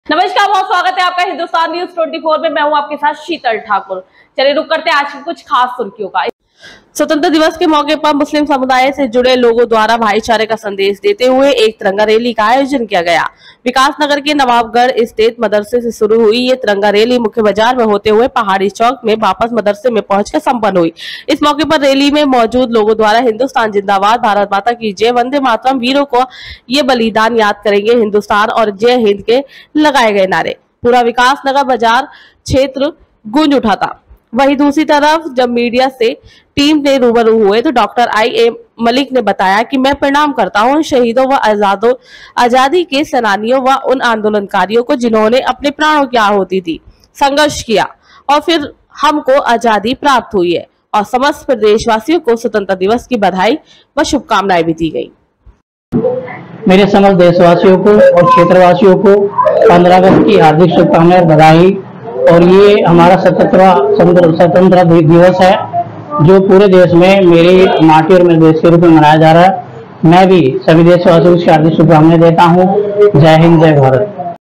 स्वतंत्रता दिवस के मौके पर मुस्लिम समुदाय से जुड़े लोगों ने भाईचारे का दिया संदेश रैली का आयोजन
इस मौके पर रैली में मौजूद लोगों द्वारा हिंदुस्तान  जिंदाबाद, भारत माता की जय, वंदे मातरम, वीरों का यह बलिदान याद करेगा हिंदुस्तान और जय हिंद के लगाएं गए नारों से पूरा विकासनगर बाज़ार क्षेत्र गूंज उठा।